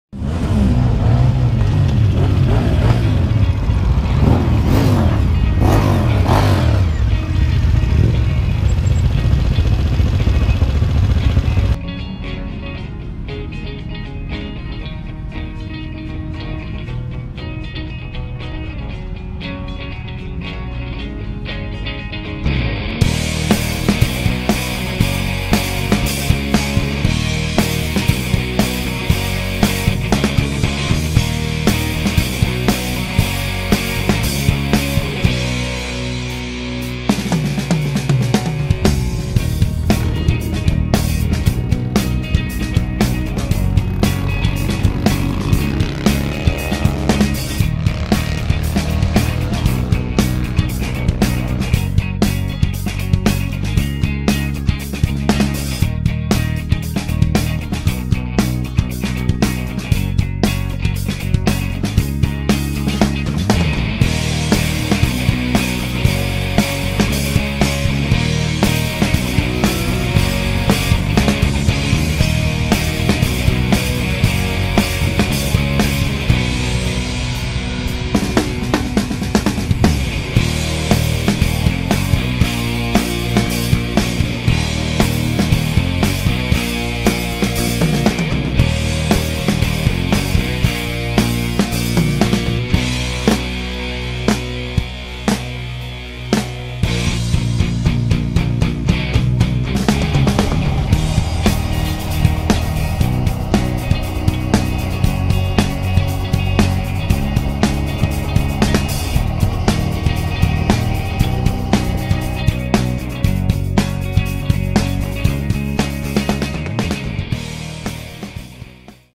Meanwhile at Squires...
A busy Thursday night at Squires Cafe at Sherburn in Elmet.